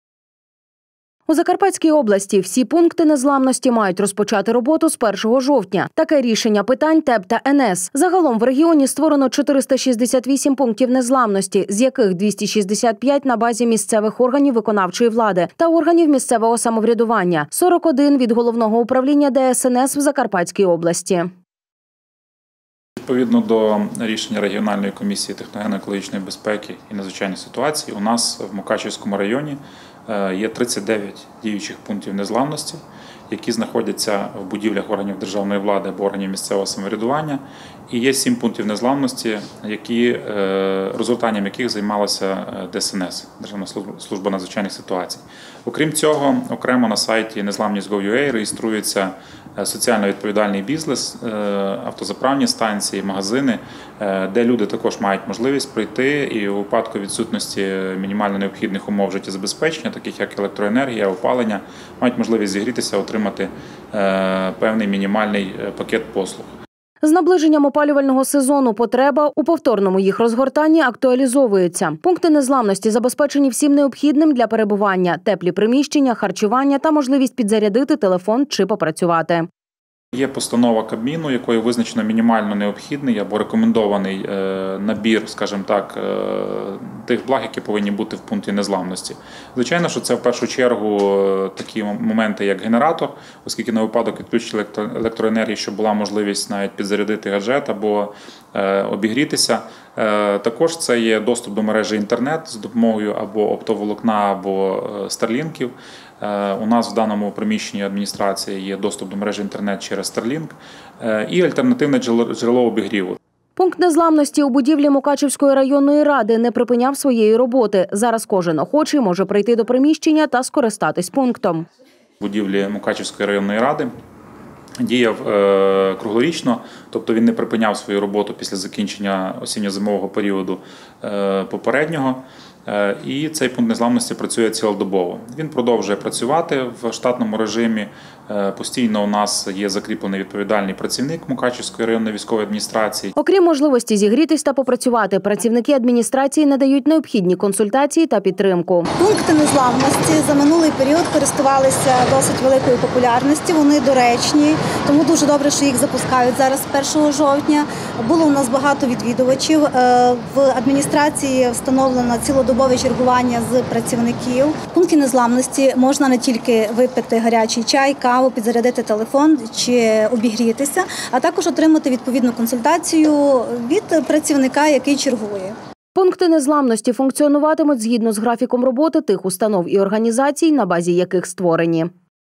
Відновлюють роботу
У Закарпатській області всі «Пункти незламності» мають розпочати роботу з 1 жовтня. Таке рішення питань ТЕБ та НС. Загалом в регіоні створено 468 «Пунктів незламності», з яких 265 на базі місцевих органів виконавчої влади та органів місцевого самоврядування, 41 – від ГУ ДСНС в Закарпатській області. З наближенням опалювального сезону потреба у повторному їх розгортанні актуалізовується. Пункти Незламності забезпечені всім необхідним для перебування. Теплі приміщення, харчування, та можливість підзарядити телефон чи попрацювати. Пункт незламності у будівлі Мукачівської районної ради не припиняв своєї роботи. Зараз кожен охочий може прийти до приміщення та скористатися пунктом. Окрім  можливості зігрітися та попрацювати, працівники адміністрації надають необхідні консультації та підтримку. Пункти незламності функціонуватимуть згідно з графіком роботи тих установ і організацій, на базі яких створені.